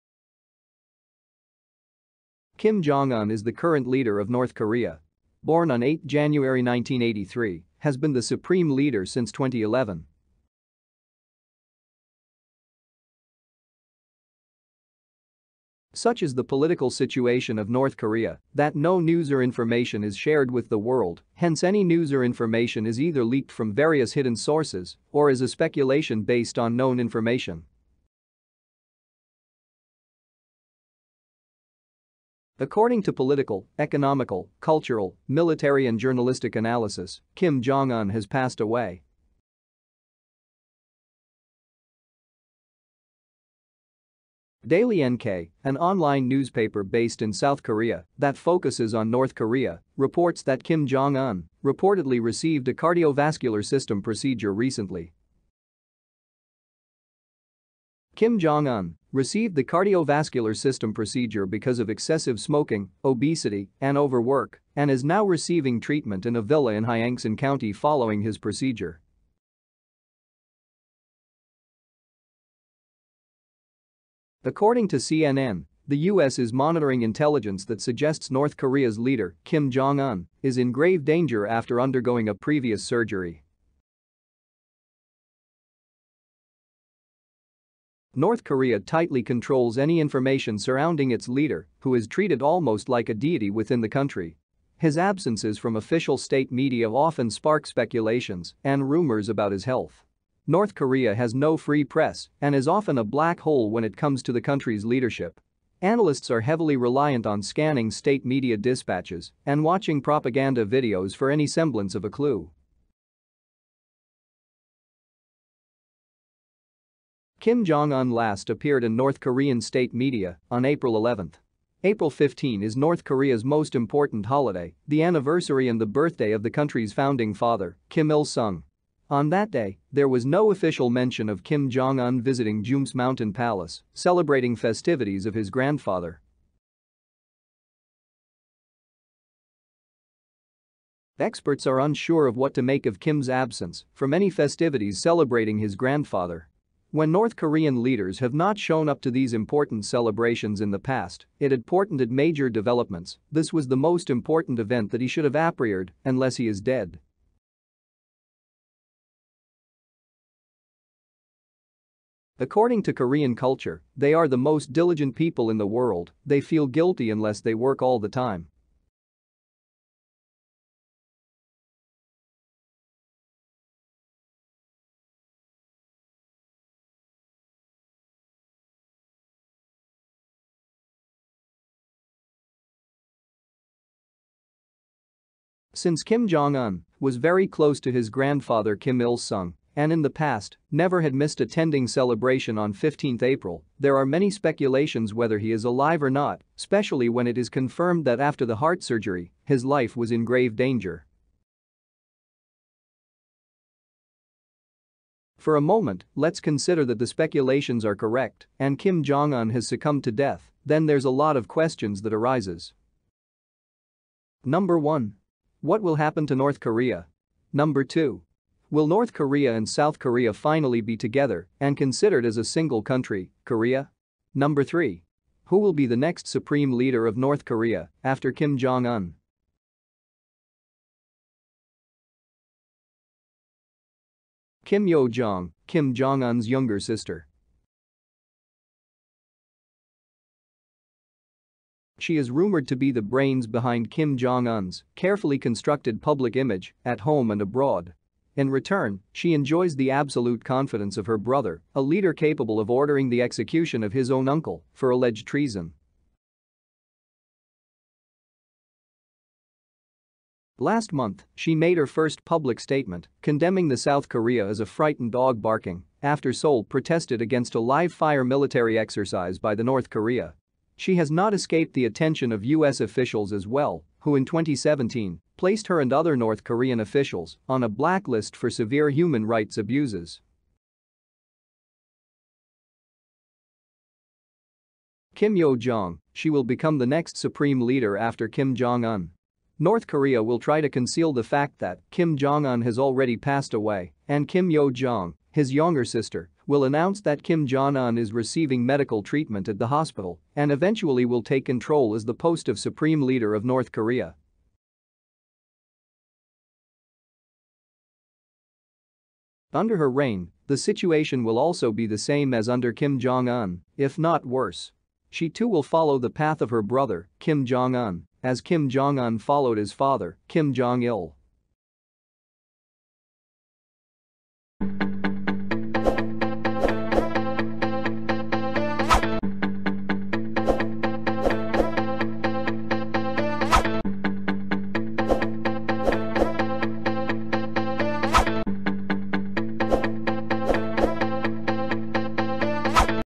Is Kim Jong Un Dead? | How Kim Jong Un Died? | North Korean Leader Died After Surgery | Kim Yo-Jong
Kim Yo-jong(yonger sister of Kim Jong Un) will be the next leader of North Korea.
North Korea tightly controls any information surrounding its leader, who is treated almost like a deity within the country. His absences from official state media often spark speculation and rumors about his health. North Korea has no free press and is often a black hole when it comes to the country's leadership. Analysts are heavily reliant on scanning state media dispatches and watching propaganda videos for any semblance of a clue.

Kim Jong Un last appeared in North Korean state media on April 11. April 15 -- North Korea's most important holiday, the anniversary of the birth of the country's founding father, Kim Il Sung -- came and went without any official mention of Kim Jong Un's movements.
Experts are unsure of what to make of Kim's absence from any festivities celebrating his grandfather. When North Korean leaders have not shown up to these important celebrations in the past, it has portended major developments. But it has also turned out to be nothing.

Hit the "Bell" icon so that you don't miss any update and hit the "Like" button to show your appreciation.